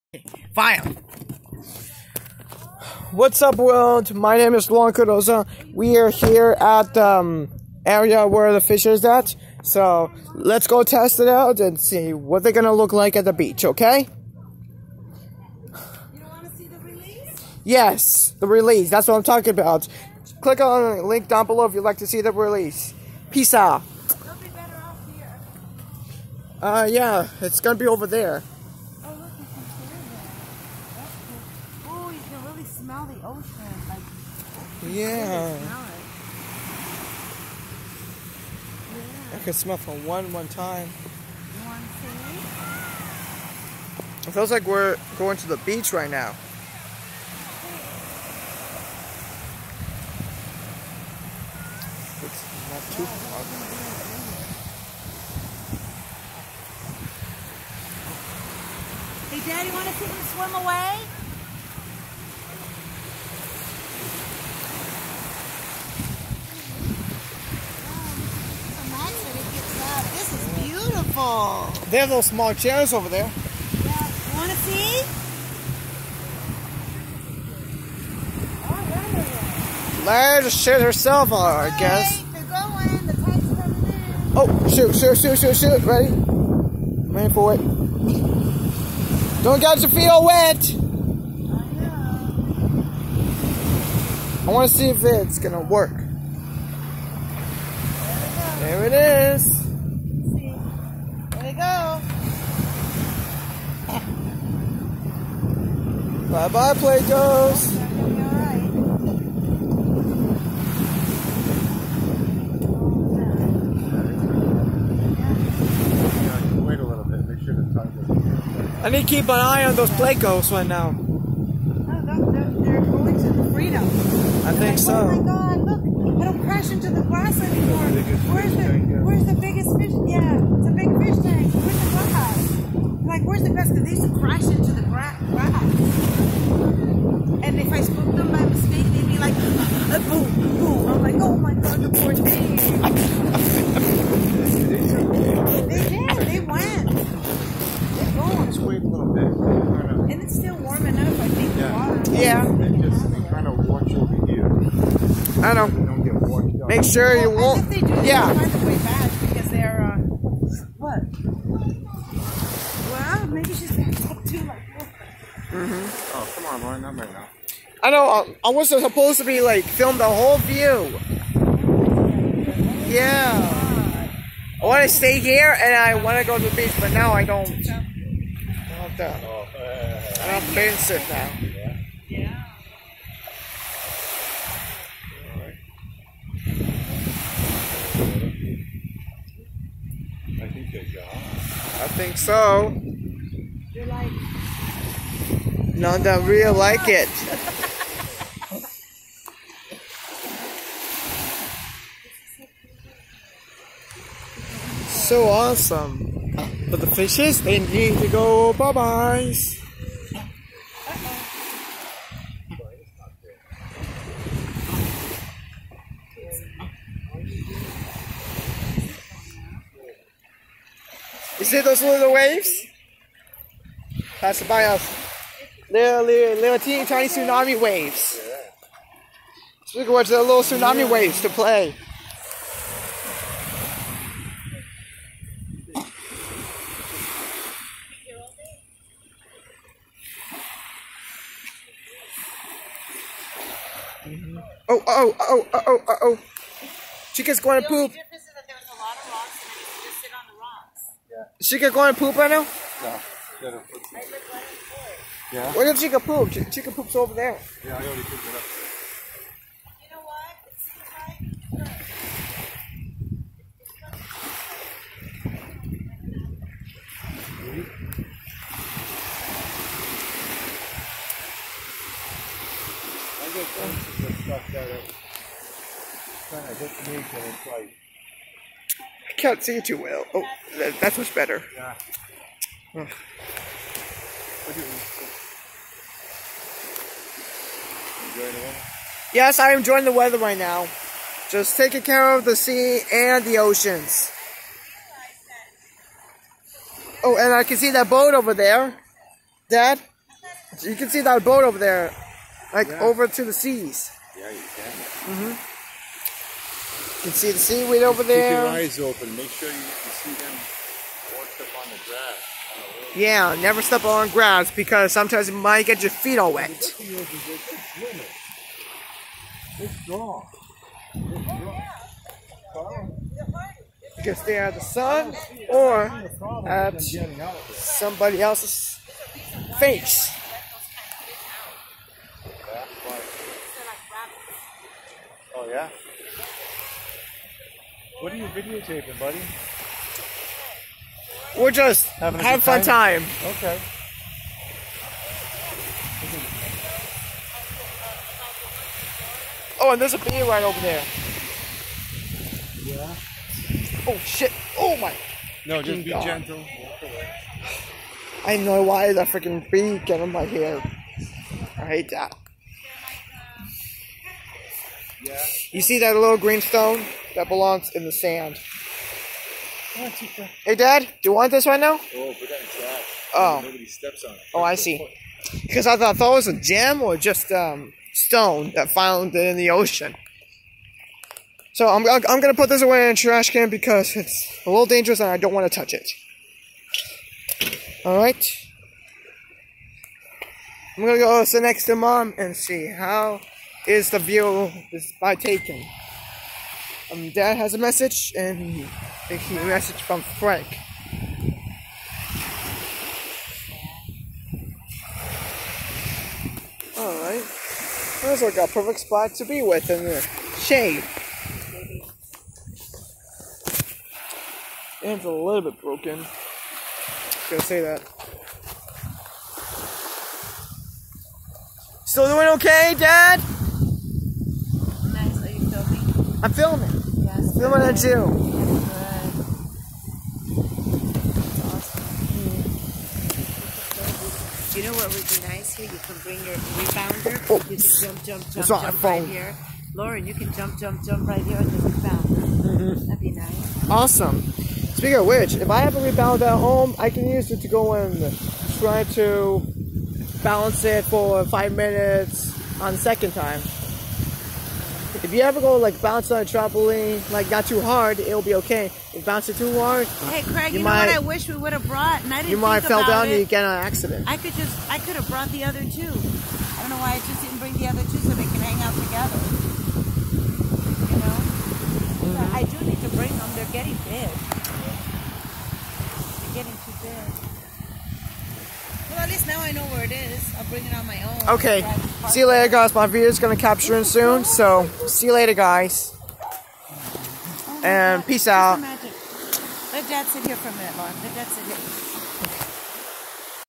Putting a black Plecostomus at the East Providence ocean lake! 🙂🌊💧💦🐟🥫🌅🏖️⛱️🏝️
I really miss those Plecostomus fish! My mom put it in the ocean lake beach to have a great home for their new habitat. I'm really really happy to say that those black Plecos are getting so huge, so big, and they all grown to be as little log sucker fish again! Say goodbye to my little old fishy black Plecostomus fish!